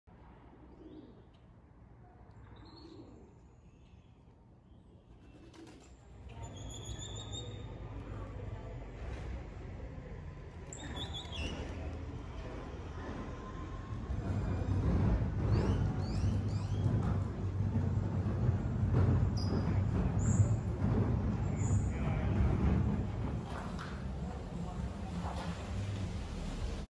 Graffiti in Getsemani, Cartegana Colombia
Exploring Graffiti arts in Getsemani neighborhood in Cartagena Colombia. This is a beautiful neighborhood with a lot of graffiti everywhere you don’t need to join a walking tour, you can just walk on your own enjoy the graffitis, it’s a very safe neighborhood.